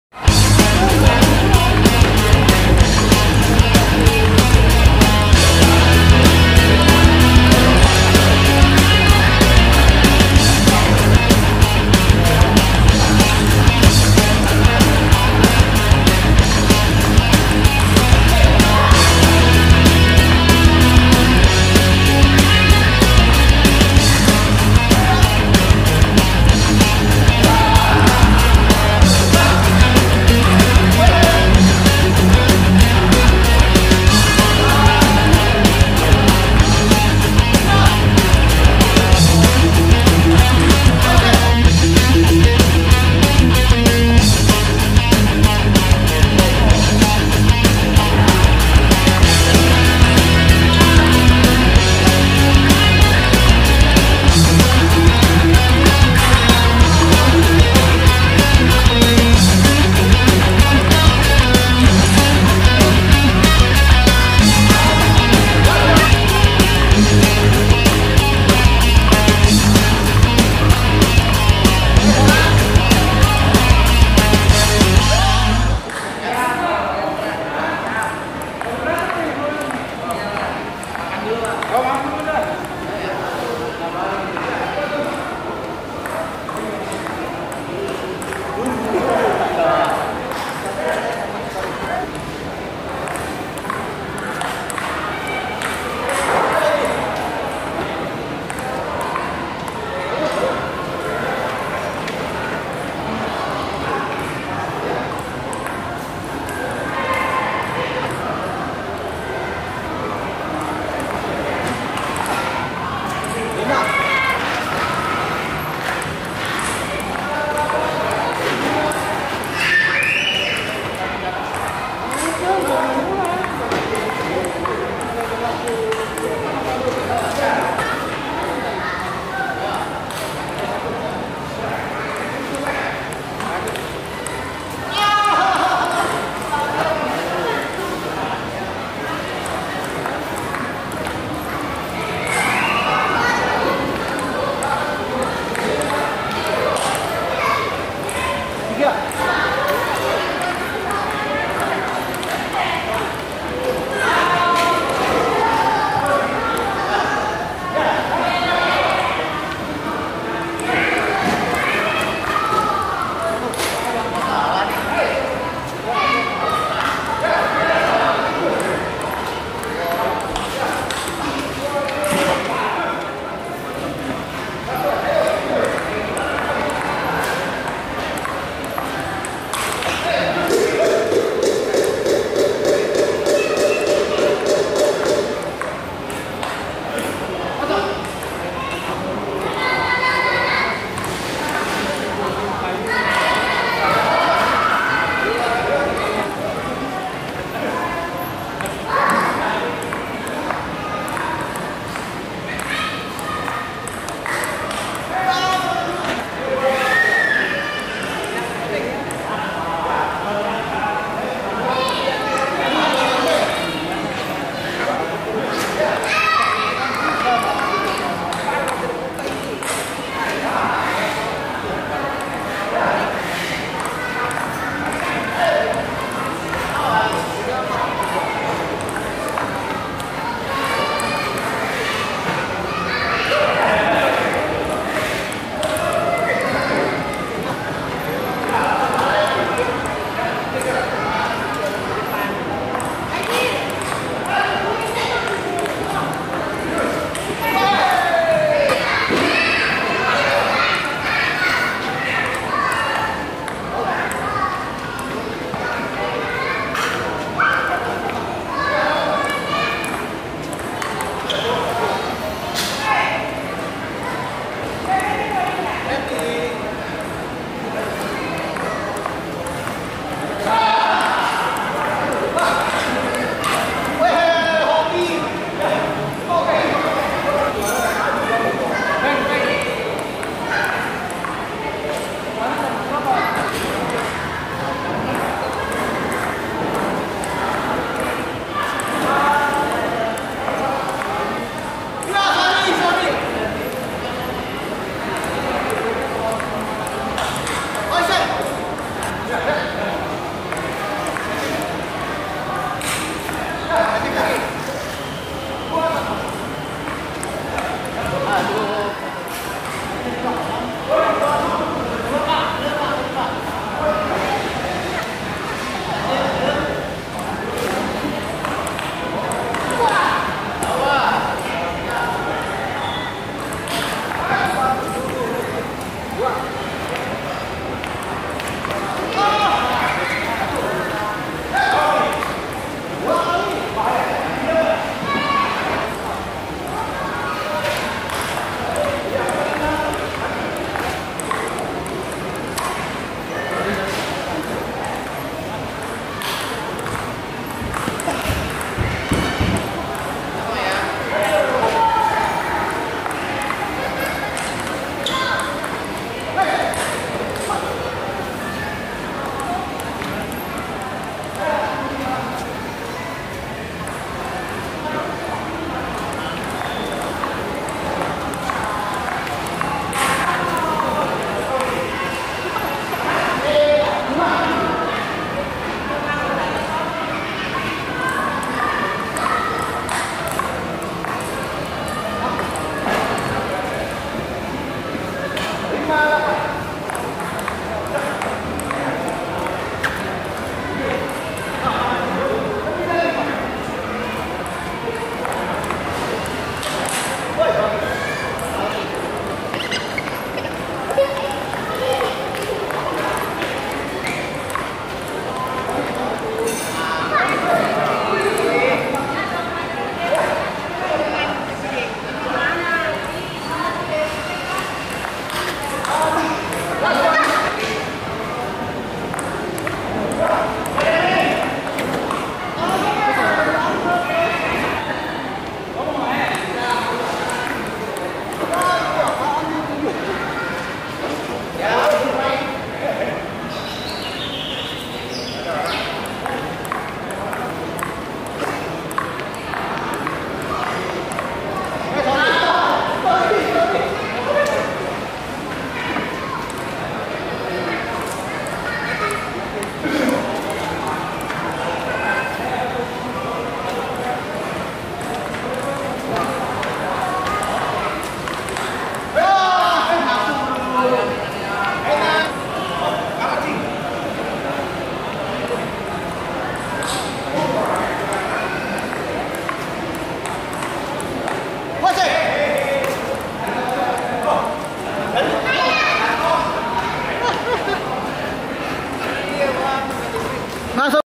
Rematch : Bintikers VS Bintikers (Apakah Cing Ridha bisa ditaklukan?)
Pertandingan kedua dengan Bang Ridha, apakah bisa ditaklukan atau malahan sebaliknya?

Terimakasih 🙏🙏🙏